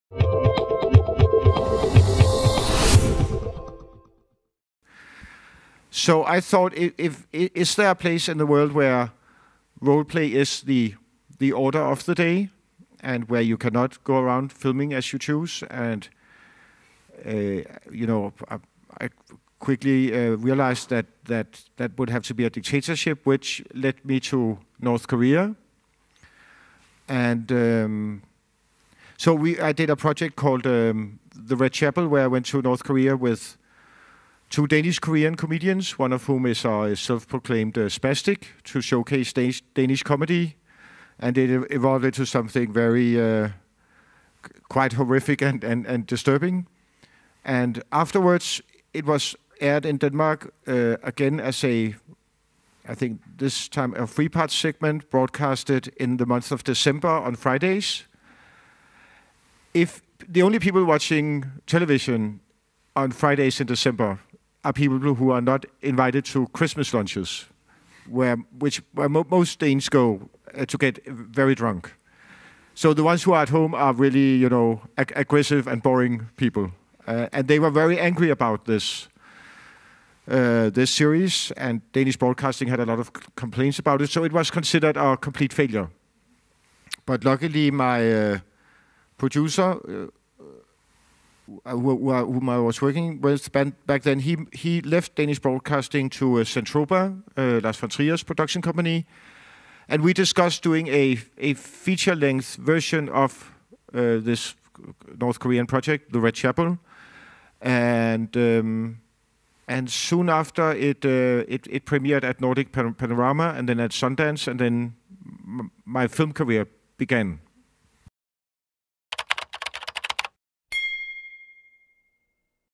MADS BRÜGGER (3) | Filmmakers Live | Filmfest München 2019
Das FILMFEST MÜNCHEN widmet dem Dokumentarfilmer, Drehbuchautoren und Journalisten Mads Brügger in diesem Jahr eine Retrospektive. Das dänische Multitalent bewegt sich virtuos im Grenzgebiet zwischen Dokumentar- und Spielfilm. Ein Gespräch über seine Ursprünge und sein explosives filmisches Schaffen.

FILMFEST MÜNCHEN is dedicating a retrospective to documentary filmmaker, scriptwriter, and journalist Mads Brügger. This multitalented Danish director is a virtuoso who explores the boundary between documentary and feature films. A talk about his origins and his explosive films.